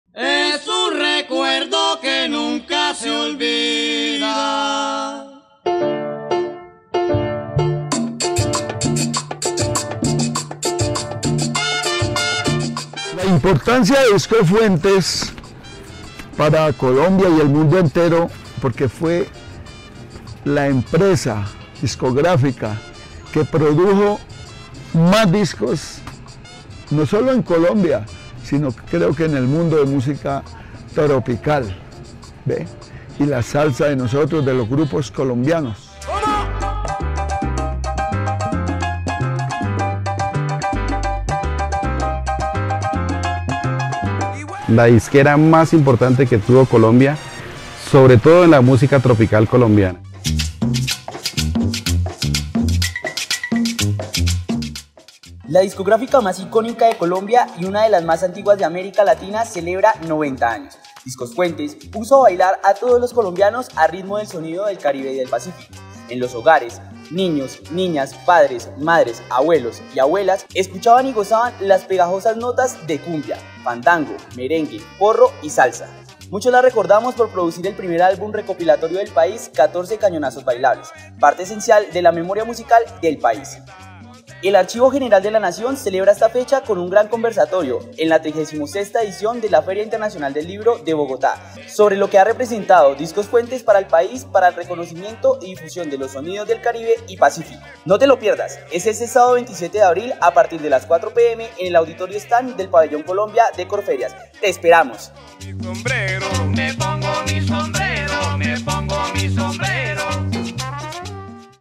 El Archivo General de la Nación es memoria sonora
Con ocasión de los 90 años de Discos Fuentes, este sábado 27 de abril a las 4 p.m. estaremos en el Auditorio estand del Ministerio de Cultura conversando sobre la importancia de esta disquera en la popularización de los sonidos del Caribe y del Pacífico y el papel del AGN en la preservación de la memoria musical de Colombia.🎵